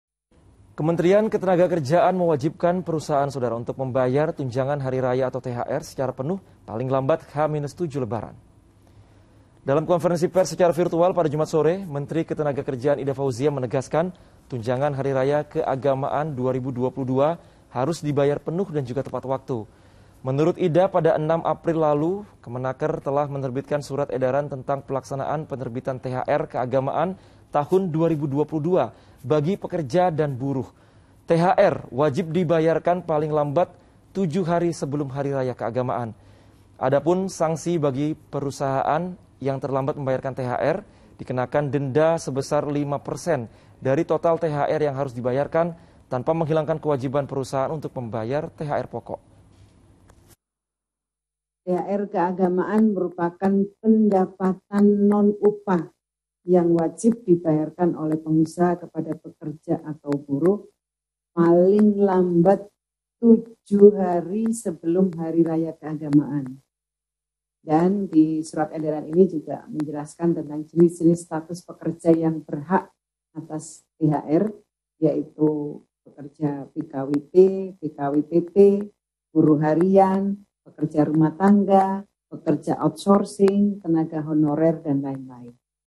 Siap-siap, THR Paling Lambat H-7 Hari Raya Idulfitri! | Liputan 6
Kementerian Ketenagakerjaan mewajibkan pengusaha untuk bayar THR secara penuh paling lambat tujuh hari raya idulfitri. Adapun sanksi bagi pengusaha yang terlambat bayar THR akan dikenakan denda.